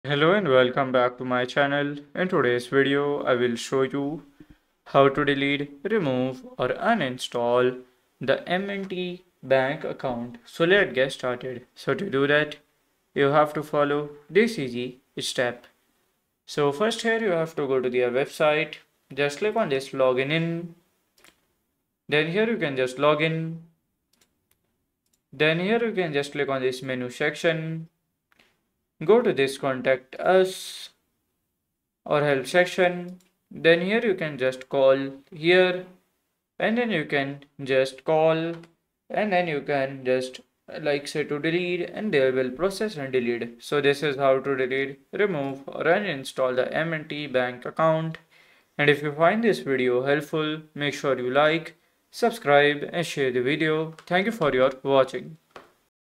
✅ How To Uninstall/Delete/Remove Manufacturers and Traders Bank Account (Full Guide)
Need to delete your Manufacturers and Traders Bank account? This video provides a step-by-step guide to help you through the process.

Key Learning Points:
• Steps to delete your Manufacturers and Traders Bank account
• Understanding account closure process
• Tips for managing bank accounts